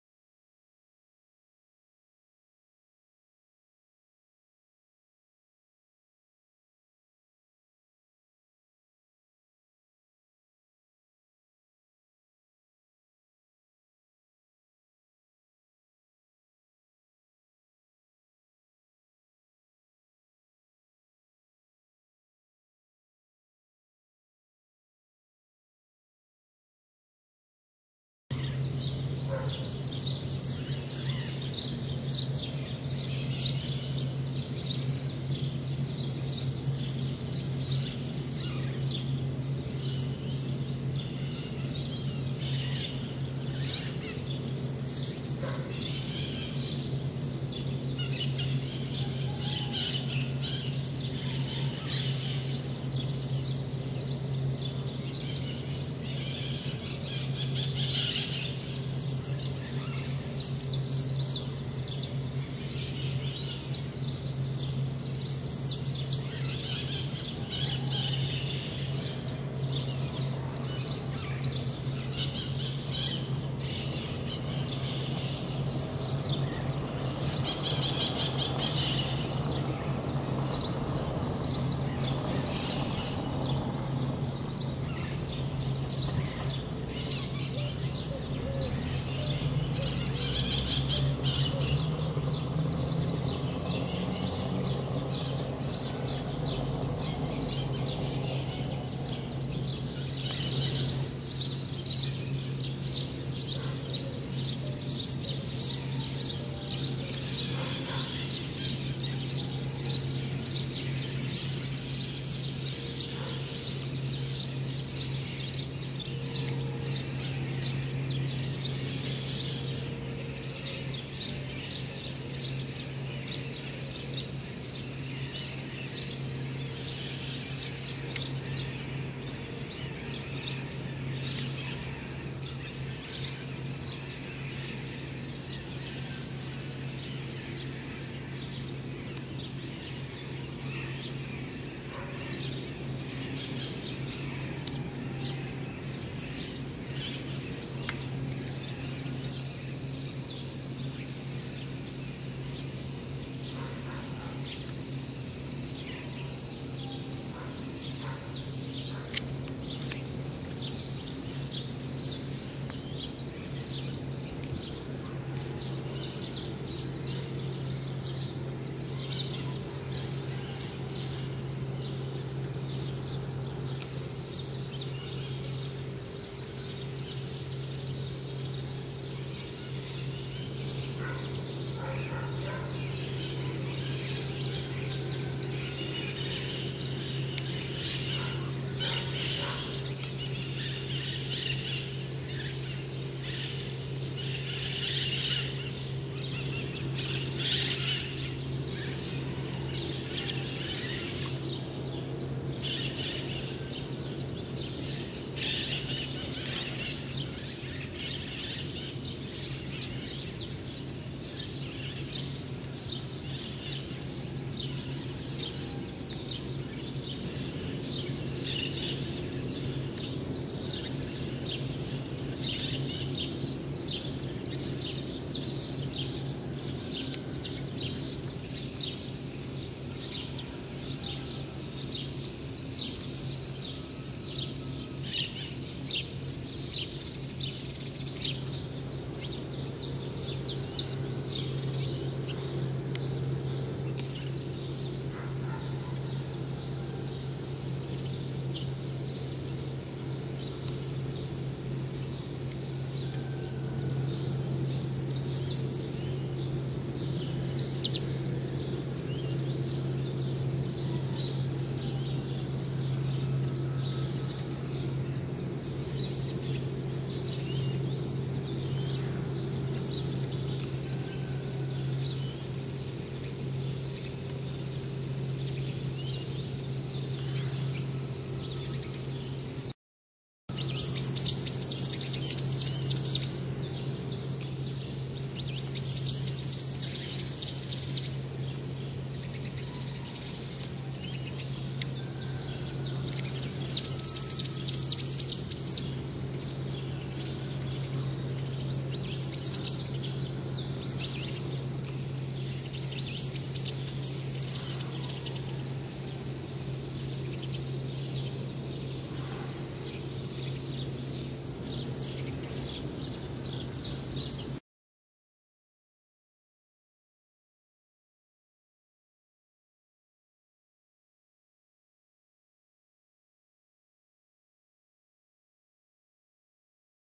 Amanhecer Euclidelândia.wmv
Amanhecer na Praça da Igreja de Santa Rita em Euclidelândia, Distrito de Cantagalo, Estado do Rio de Janeiro. Janeiro de 2012.